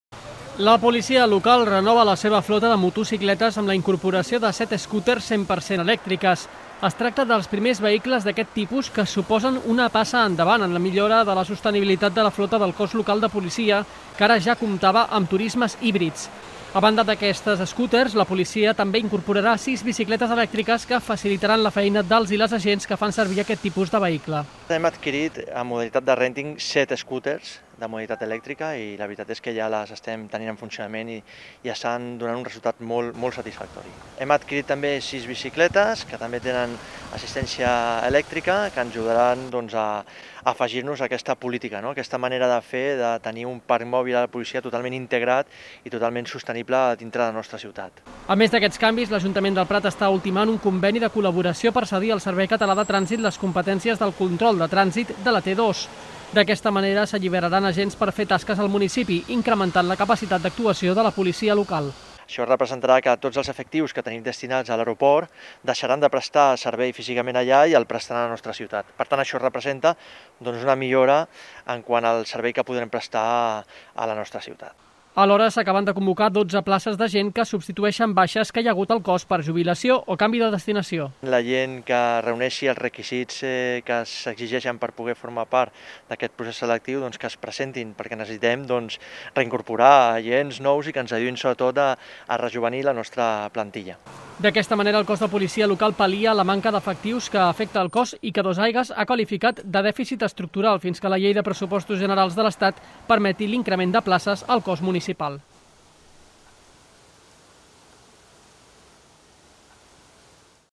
La Policia Local fa una passa endavant en l'electrificació de la seva flota
Amb la incorporació de 7 scotters 100% elèctriques i 6 noves bicicletes elèctriques el cos de policia local millora la sostenibilitat dels seus vehicles, que fins ara comptaven amb turismes híbrids. A banda d'aquesta novetat, la Policia Local ha obert una convocatòria de 12 places i recuperarà els efectius destinats a la T2 per fer tasques al municipi.